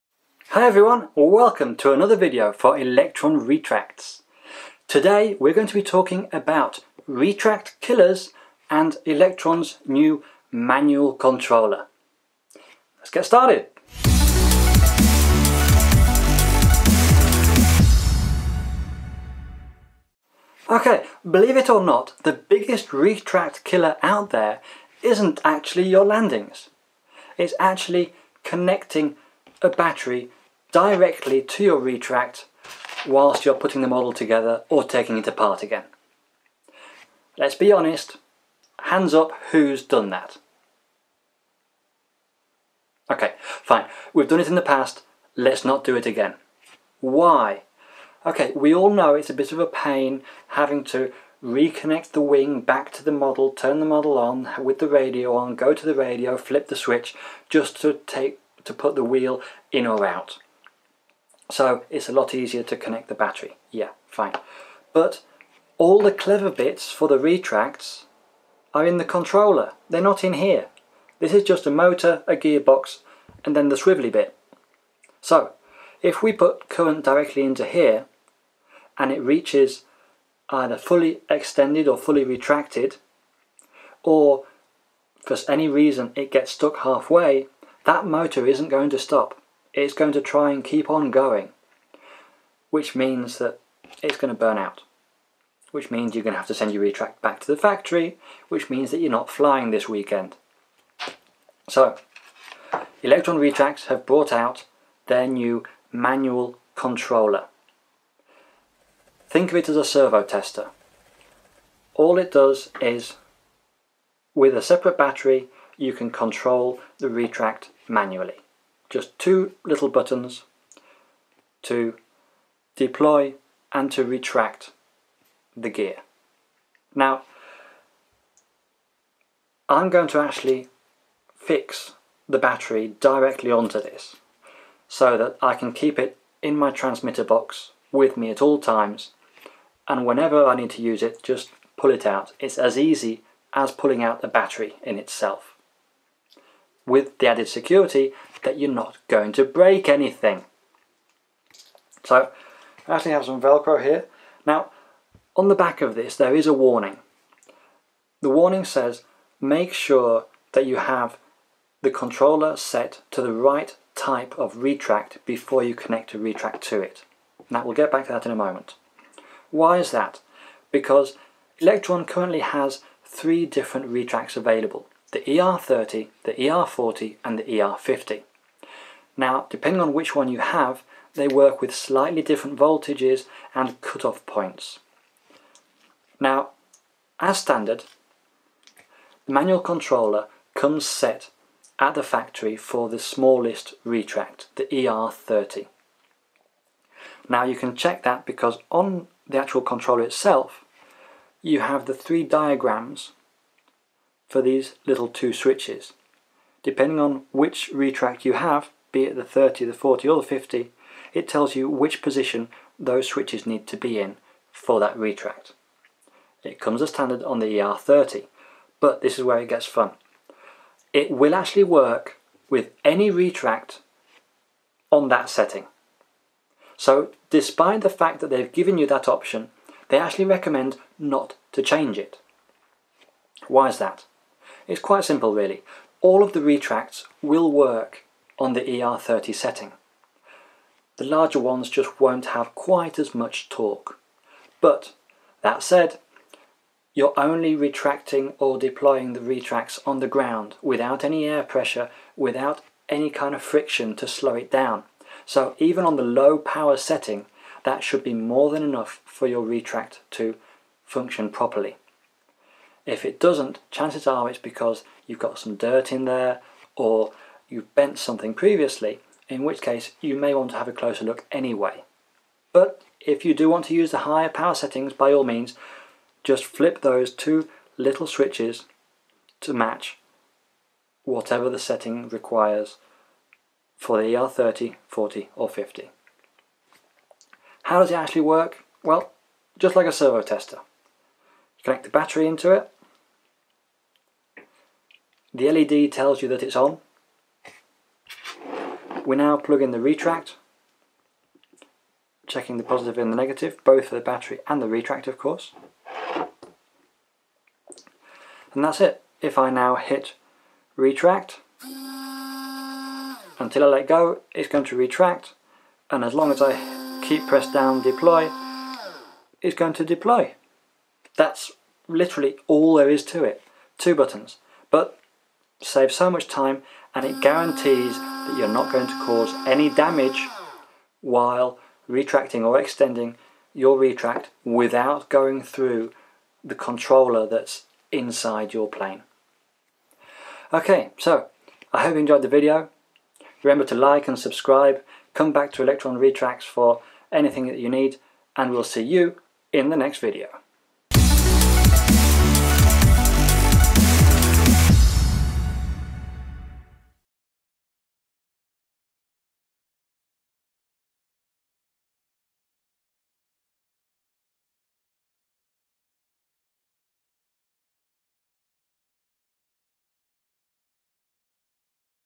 Electron Retracts - Manual Controller
Presentation of the Electron Retracts Manual Controller, as well as why it should be used instead of a direct battery and how to use it.